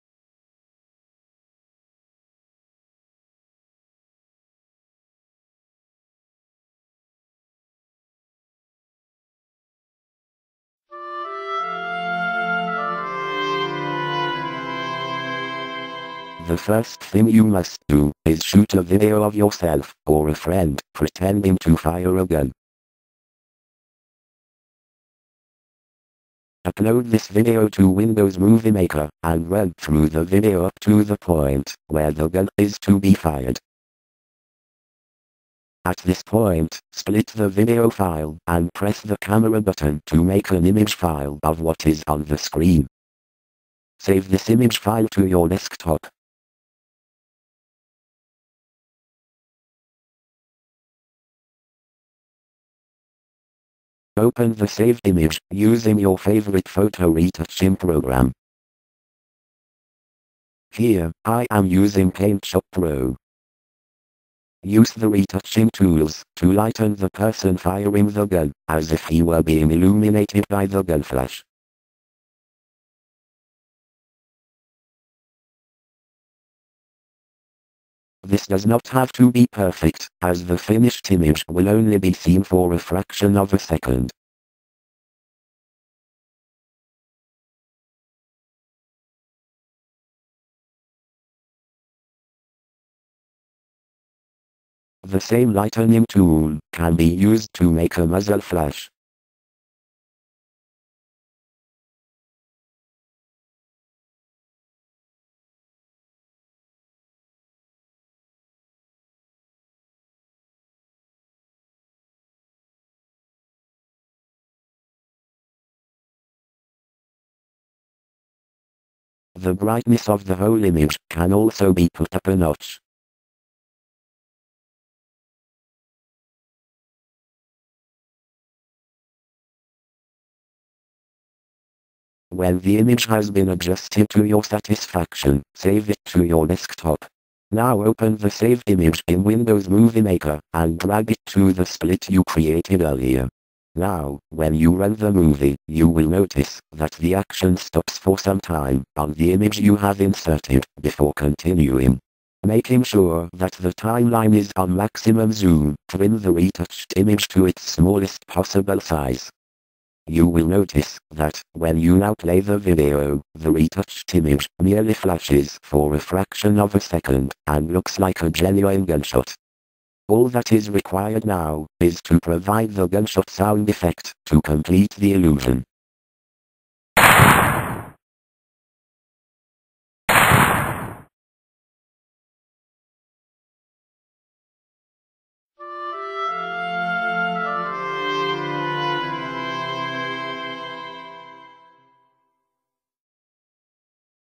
WMM Gun Effect Tutorial
I have received a number of emails asking how I did the gun effect, so I have knocked up this little tutorial to show how it was done.